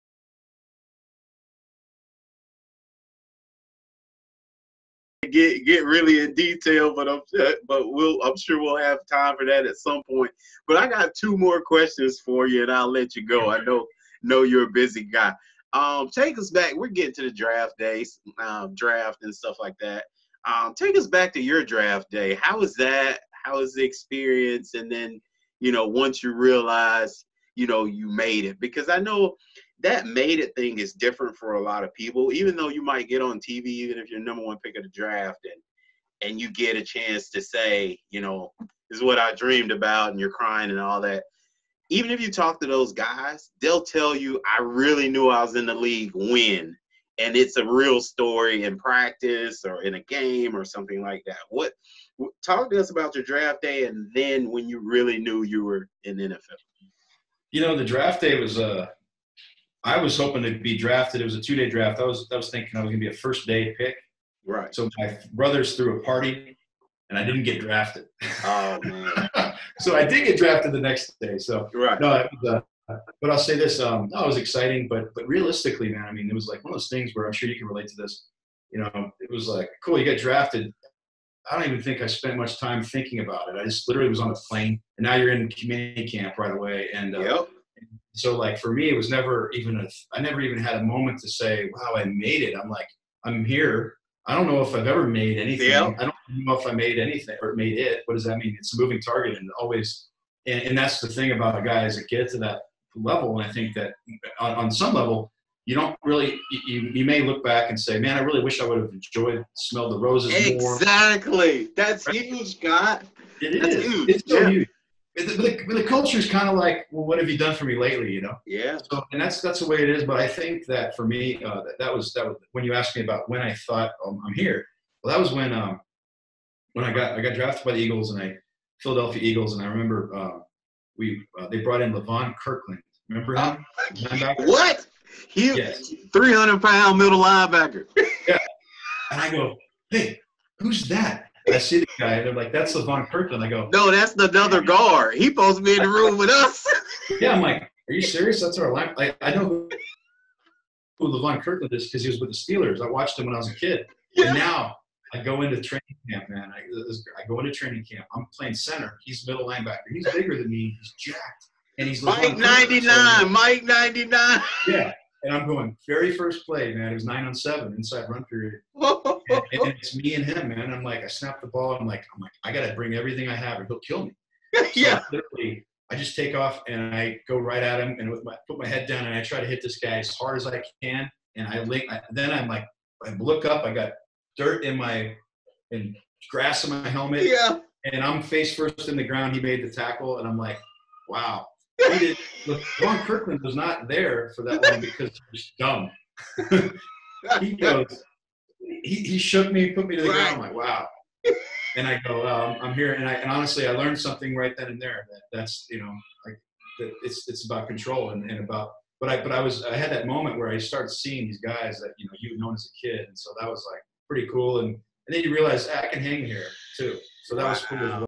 Scott Peters | Early Days in the League
An excerpt of a conversation between Chipotle Training Table Market Manager and NFL Vet Kevin Lewis and Scott Peters, NFL Vet, Cleveland Browns Offensive Line Assistant, and Founder/CEO of Tip of the Spear Football.